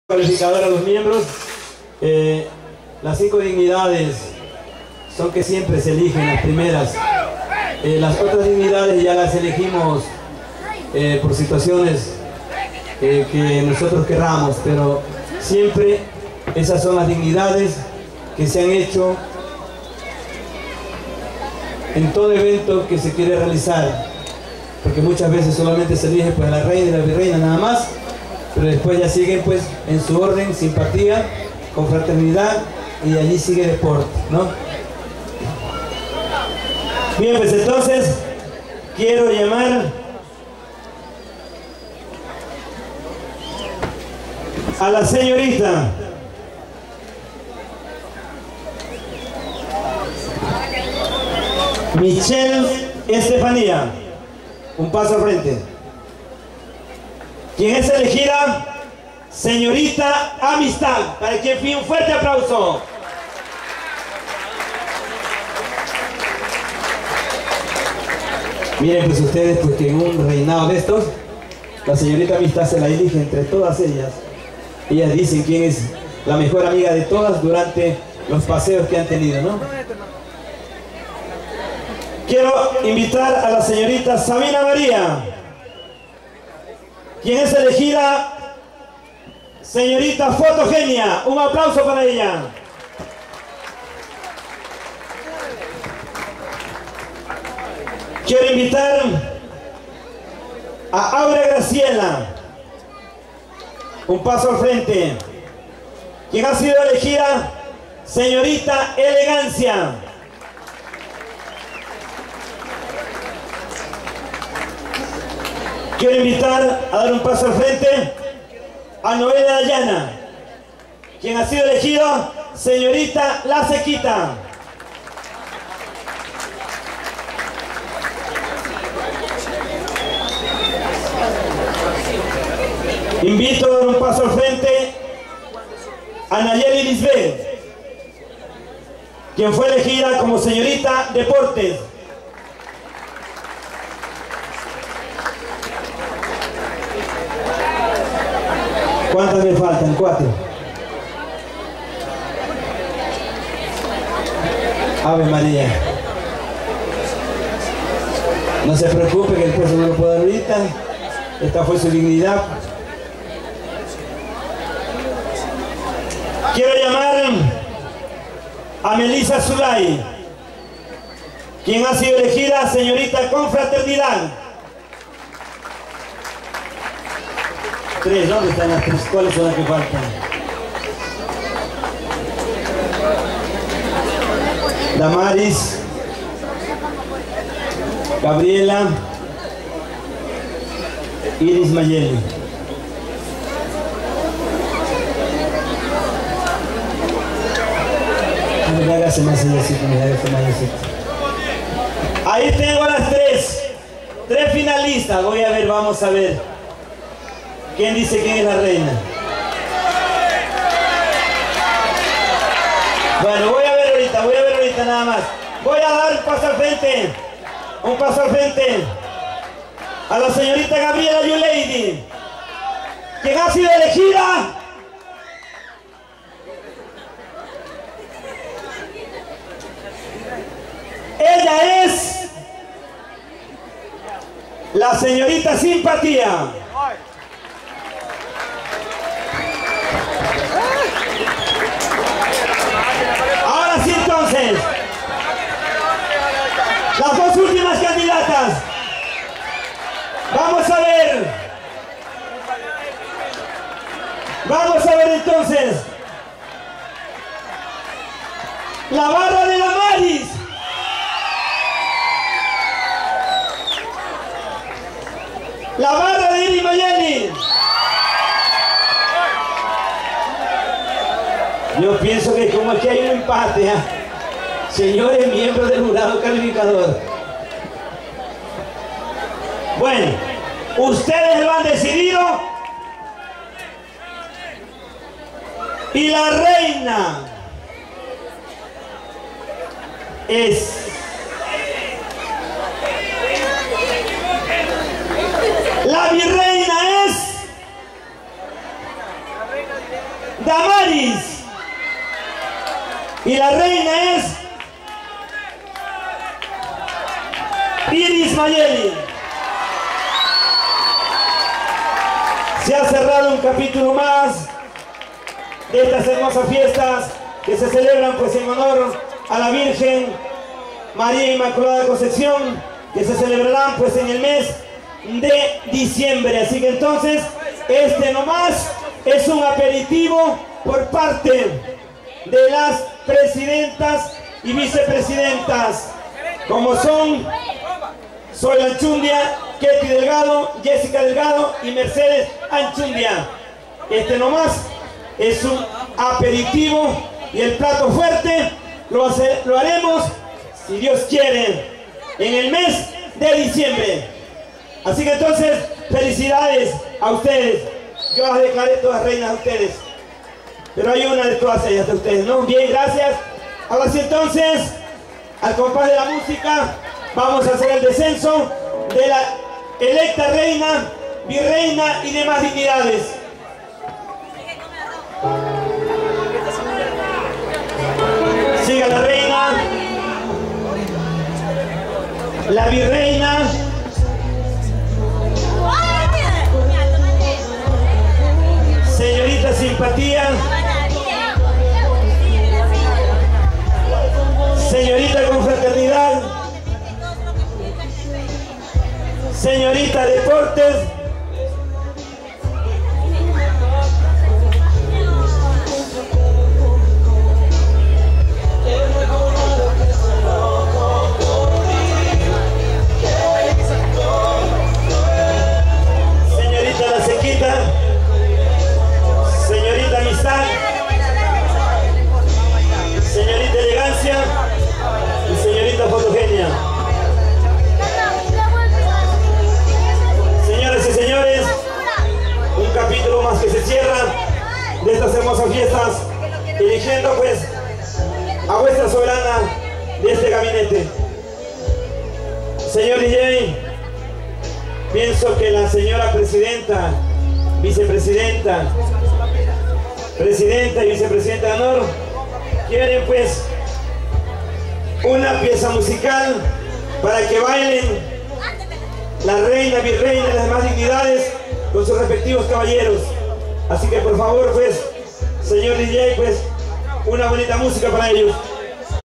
Candidatas a Reina para la fiesta Maria Inmaculada
este es el video de Elecion a Reina para las Fiesta de Maria Inmaculada en el mes de Diciembre . filmaciones Anchundia
para todos los amigo en Guayaquil en el portin  y para los amigos en Pepa de Huso y serro Azul